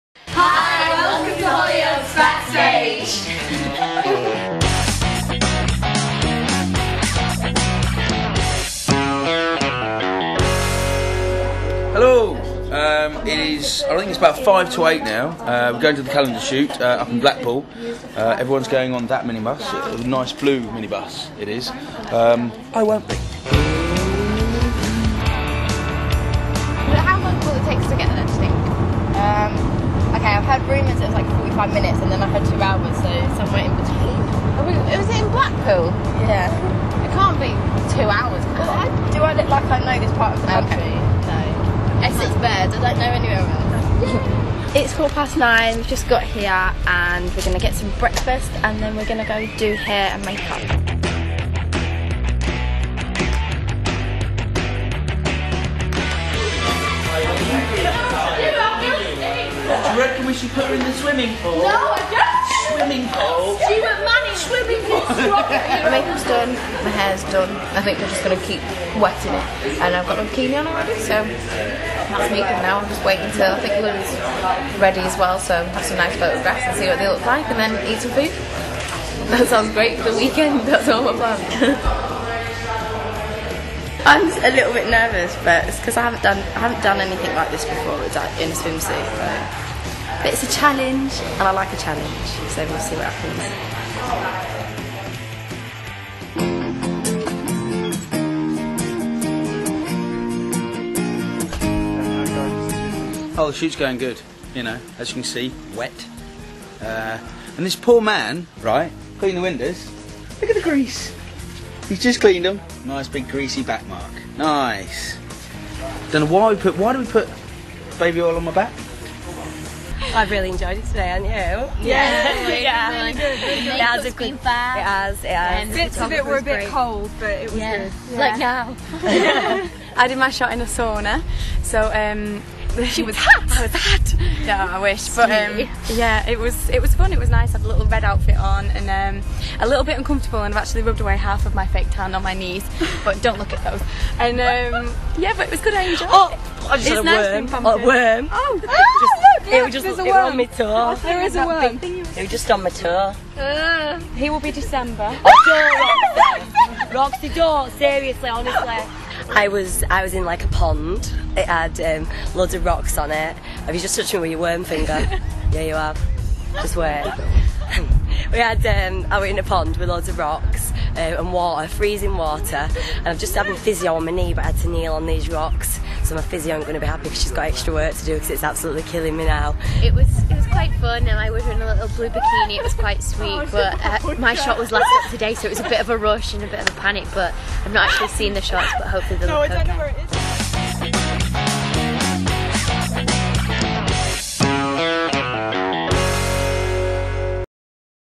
Hollyoaks Backstage - 2008 Calendar Shoot (2)
Here's backstage video looking behind the scenes of the 2008 Hollyoaks Calendar Shoot.

Part 2 is with Leah Hackett (Tina), Stuart Manning (Russ),Loui Batley (Sarah Barnes), Emma Rigby (Hannah), Roxanne McKee (Louise), Carley Stenson (Steph), Jennifer Metcalfe (Mercedes) & Nathalie Emmanuel (Sasha).